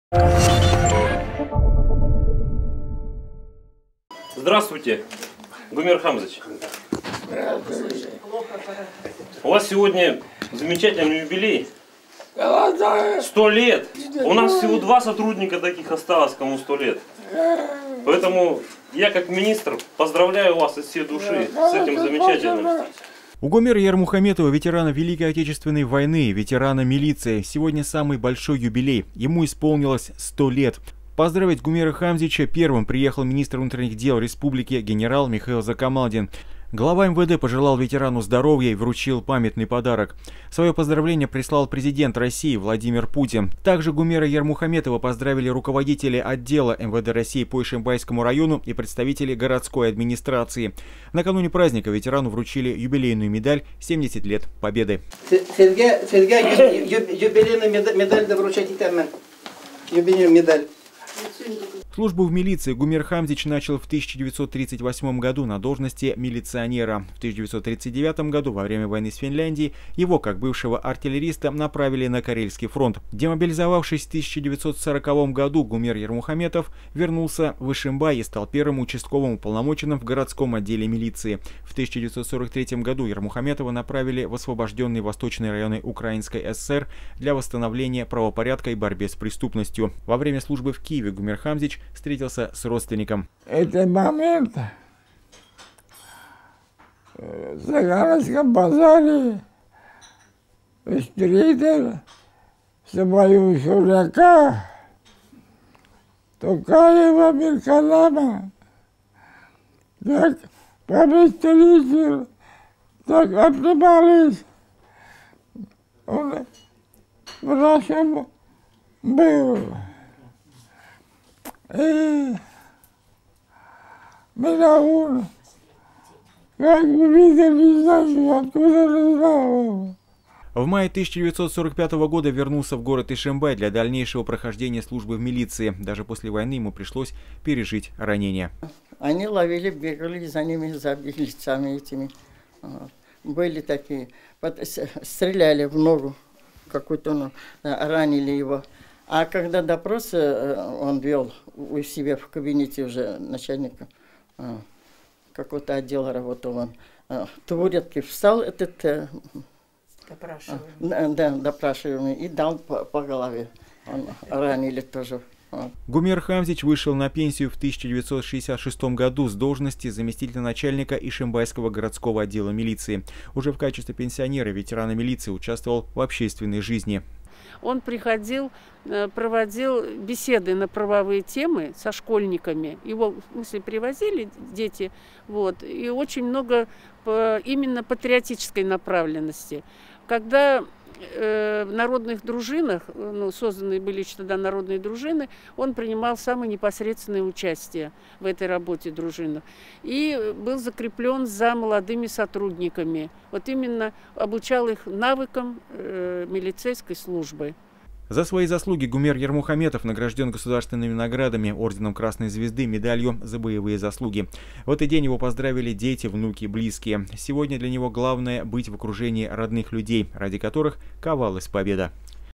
100-ый юбилей ветерана
Ветерану милиции и Великой отечественной войны Гумеру Ярмухаметову из Ишимбая исполнилось 100 лет. Его с этим юбилеем поздравил министр внутренних дел Башкирии Михаил Закомалдин.